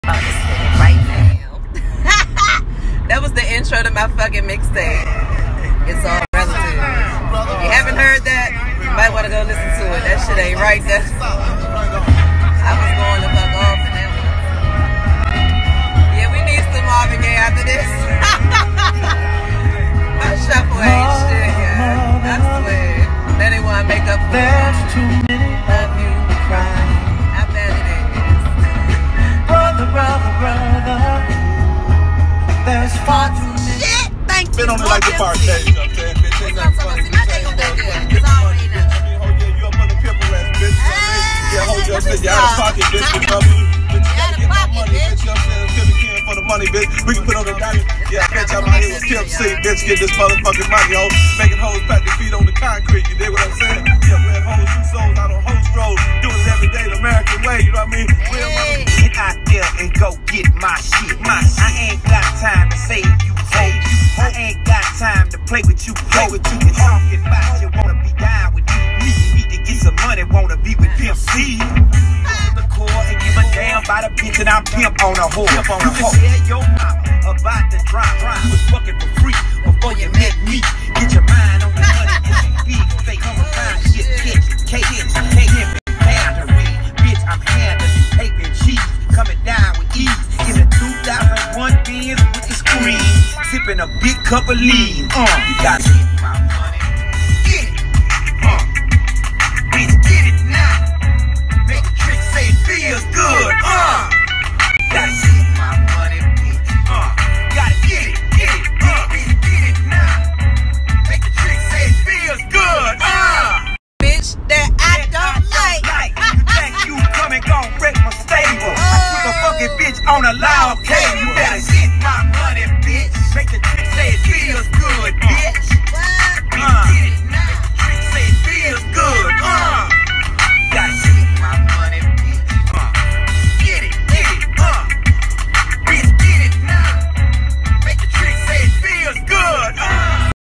Vlogging with Lady Cee / Episode 267- Marvin Gay to Pimp C shuffle 🔀 on 😜
In this episode of vlogging with Lady Cee I’m listening to my crazy shuffle and trying to let it just ride, but it’s all over the place😭😂😂😂

Thank you for your support as it is always appreciated! 🙏🏽🙏🏽🙏🏽🙏🏽

Click the link below to hear Lady Cee’s catalogue of music 🎶